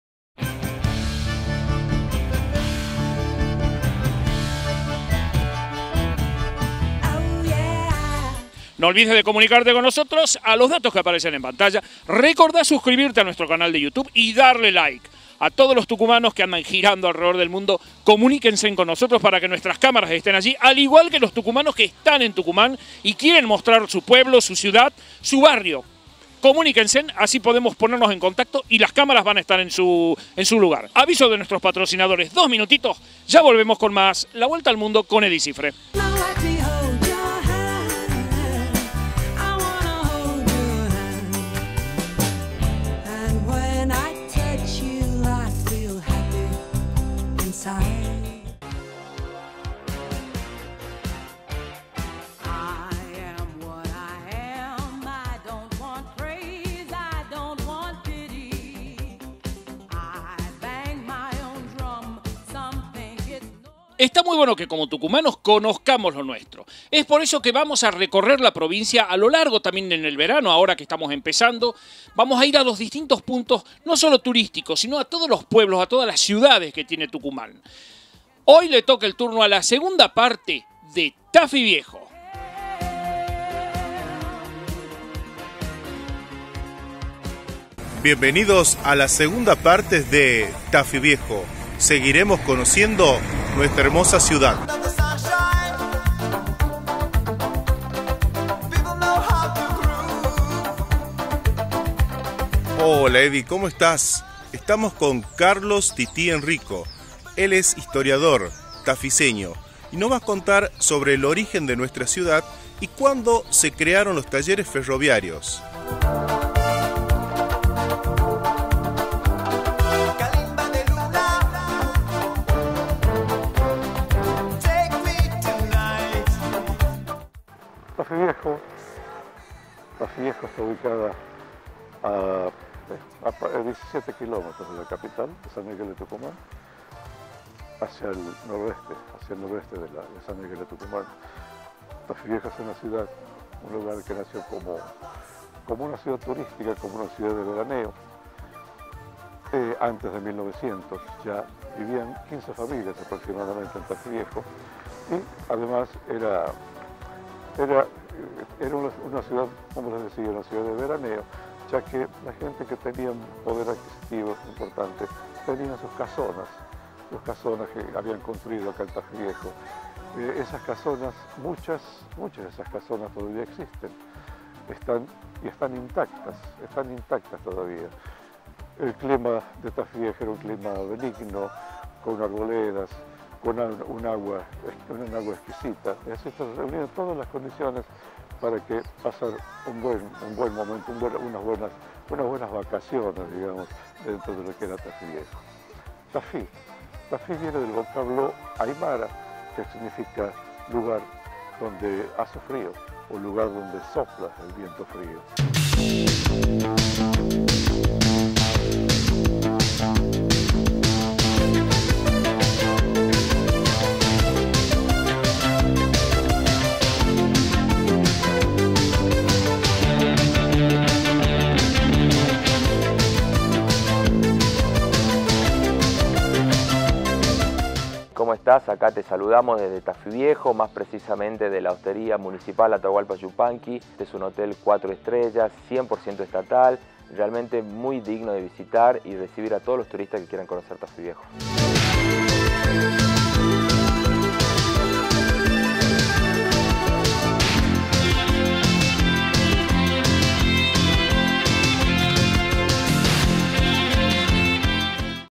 TUCUMANOS POR TAFI VIEJO
Visitamos Tafí Viejo, localidad al norte de la provincia de Tucumán.
Conocida por ser uno de los mayores exportadores de limón del mundo.
Un departamento pujante en la provincia, con una historia rica.
Se encuentran los talleres ferroviarios, que supieron ser los más grandes de sudamérica.